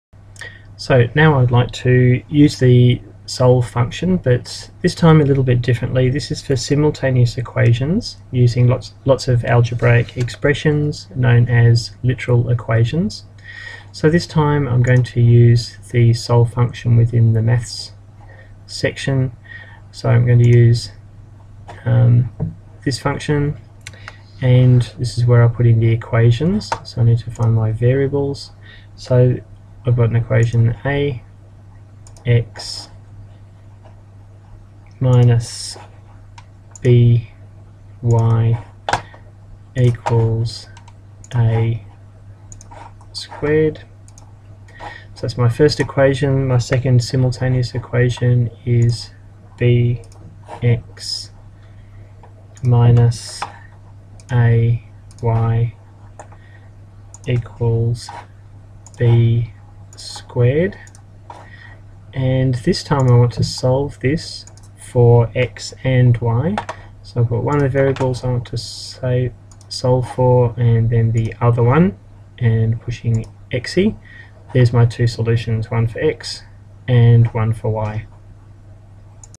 literal simultaneous equations
solving literal simultaneous equations using casio classpad 2